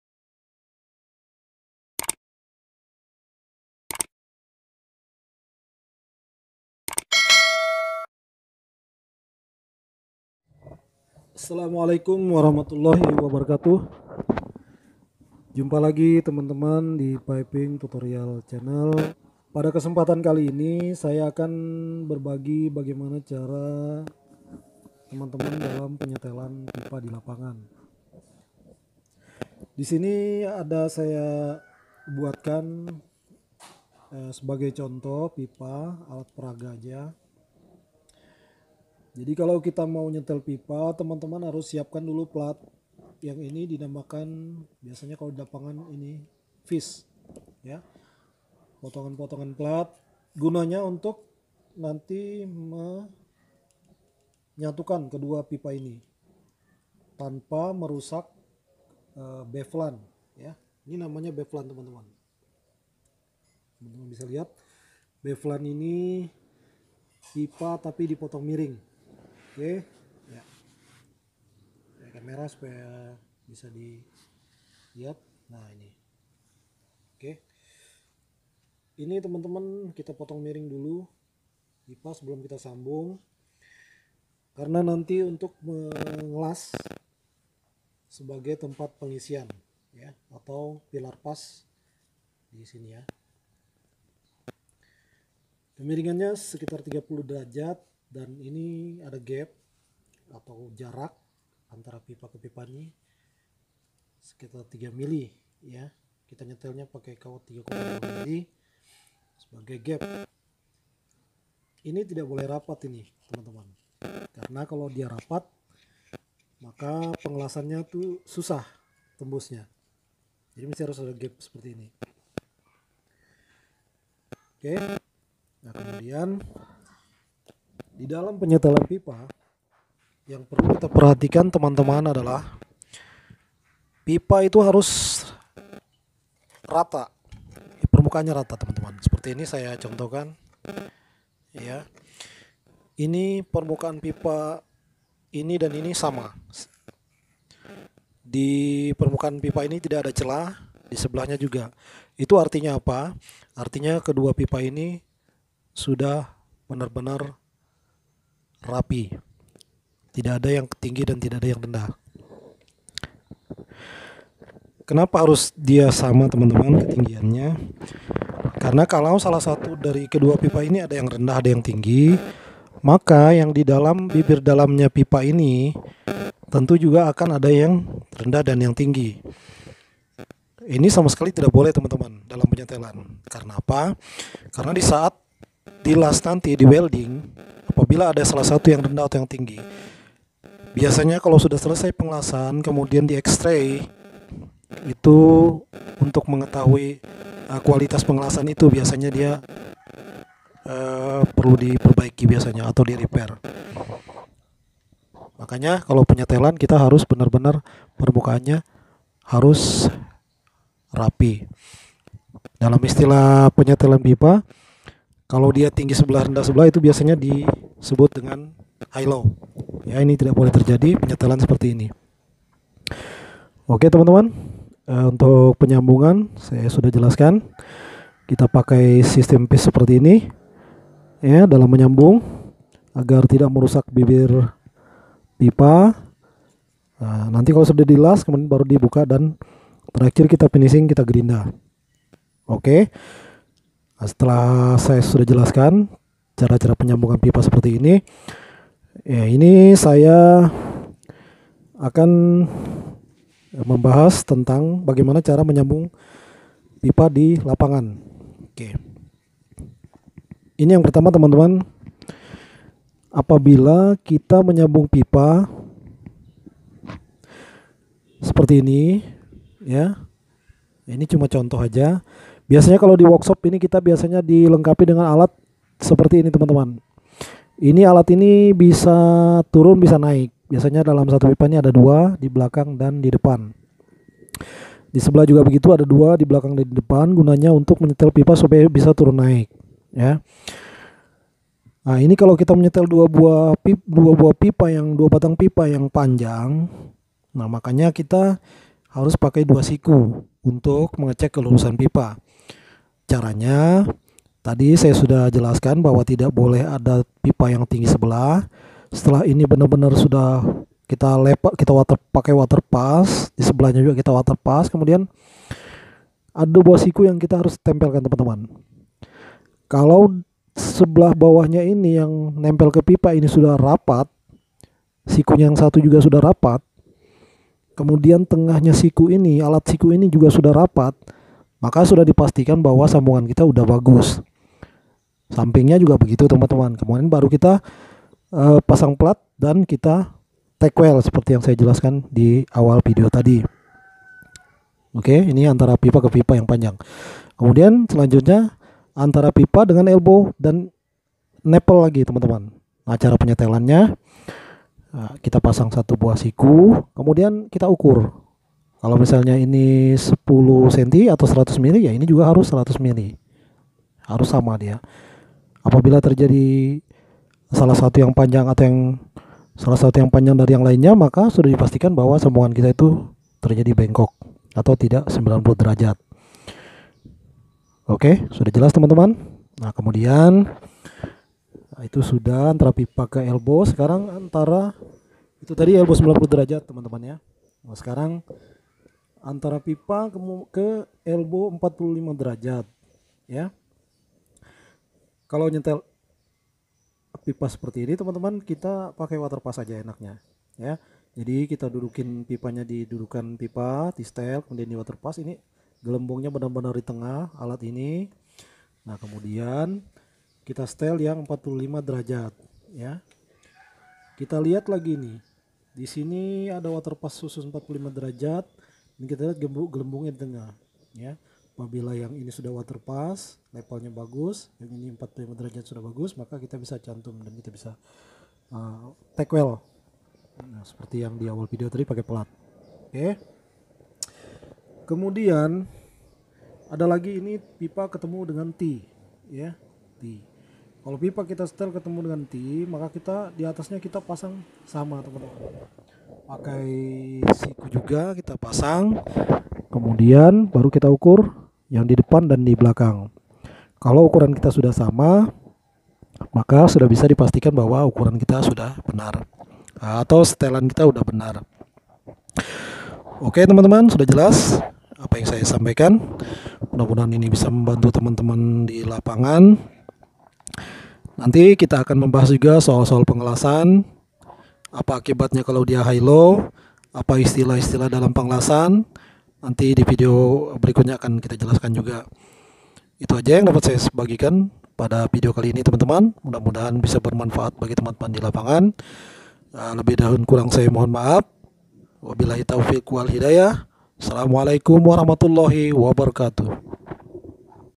Cara menyambung pipa di lapangan
Video ini menjelaskan bagaimana cara menyetel atau menyambung pipa yang benar..
ikuti terus video saya terutama tentang pekerjaan konstruksi pipa